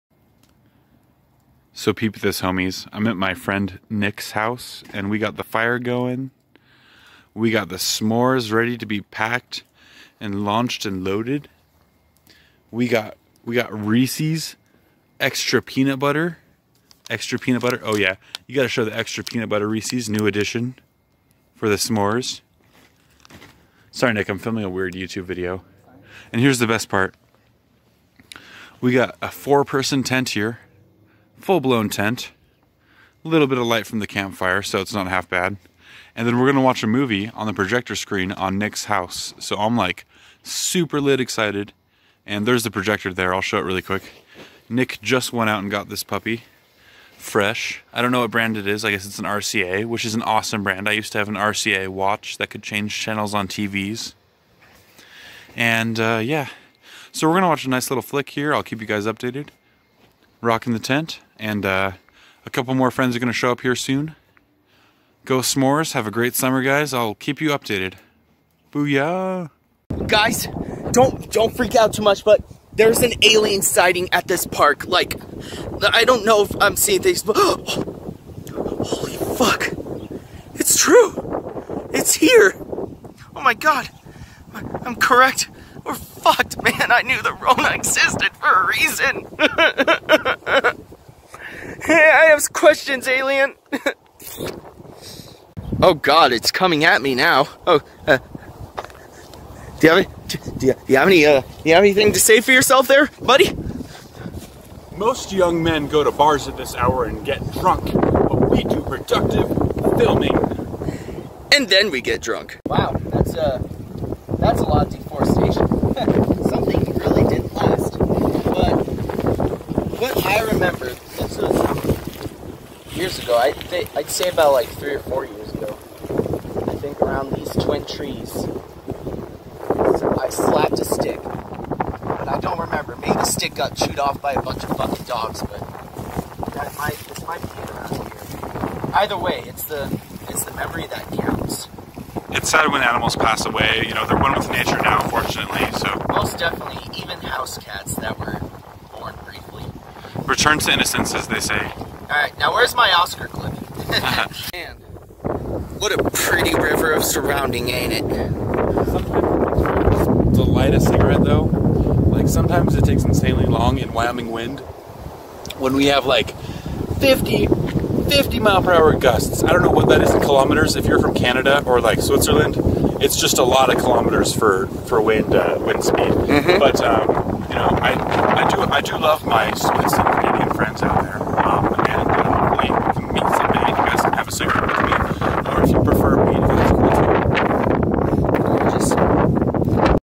S'mores Camping Adventure and Platte River Park Adventure Windy
1st sector of the video I'm enjoying getting ready for a fun slumberparty at my pal Nick's house, then second part I'm trying to film some amazing shenanigans with Jack during a windy day that makes us barely audible yet good great times !!

Please enjoy,  many many silly or exploring videos to come for my awesome loyal subscribers.

Woo.